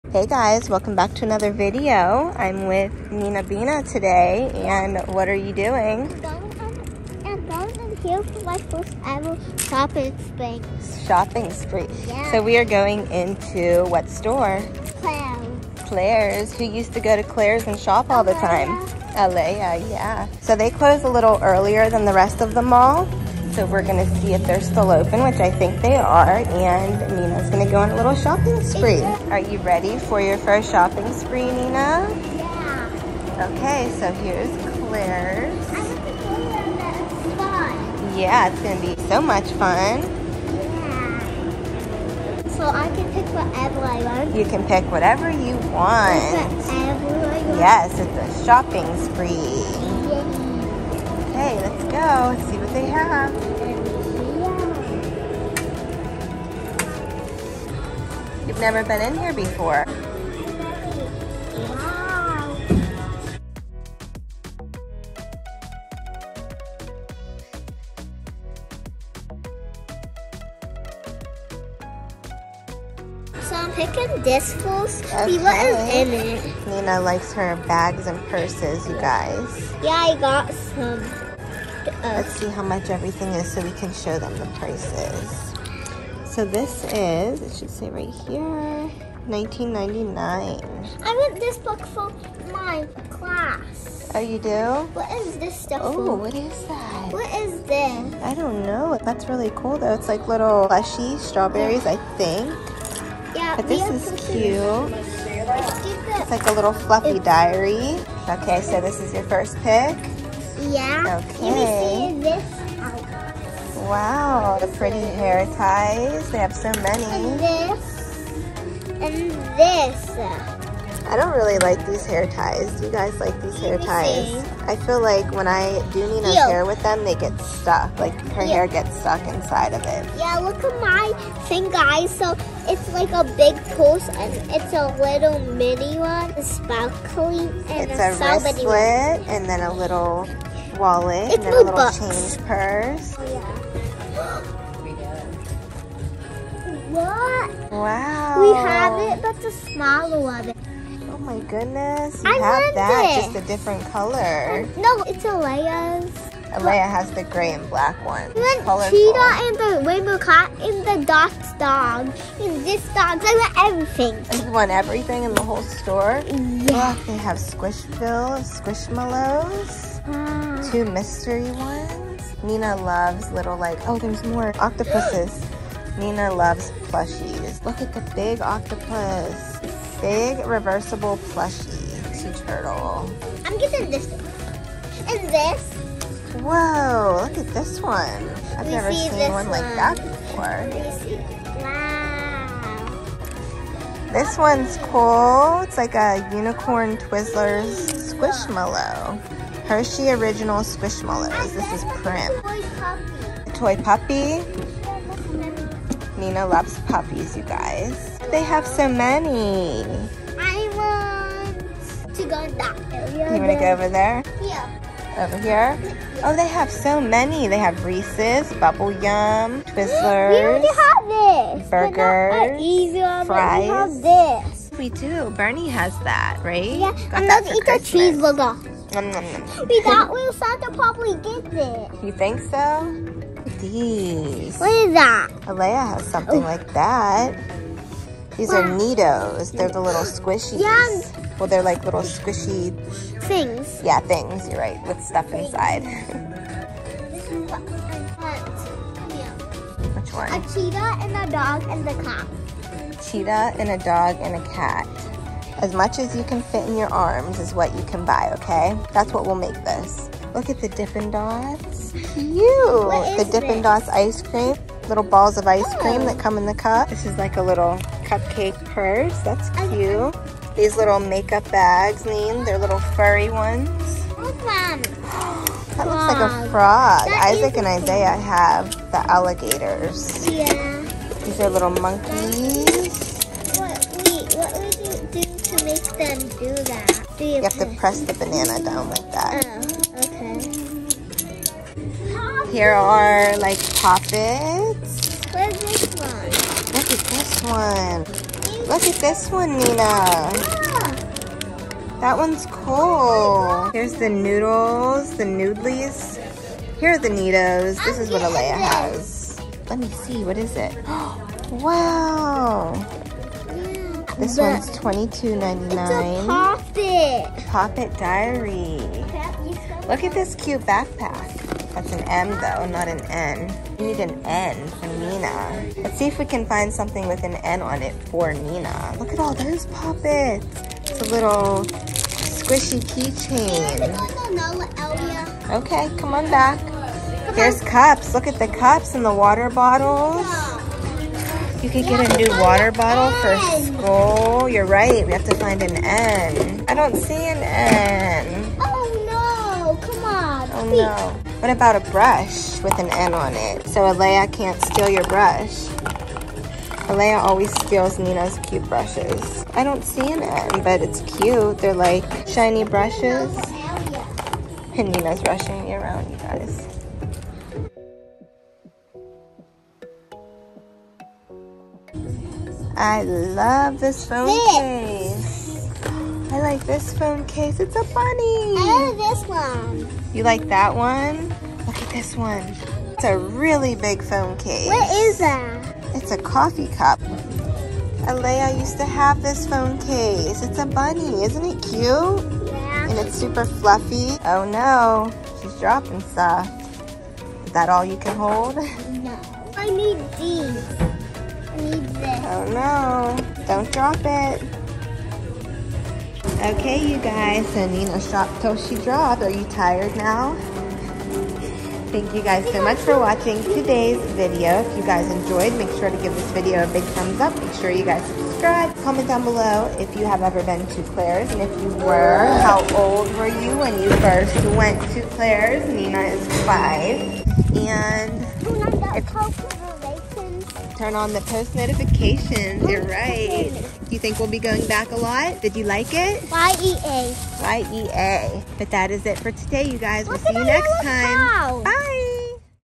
Shopping Spree at the MALL! Buying Her Whatever She Can Carry! 🛍
Mya's Instagram: myagoetz_

Mya's tiktok: myagoetz

Alayah's TikTok: alayahlmao

Shop our Poshmark Closet: Laceymyalayah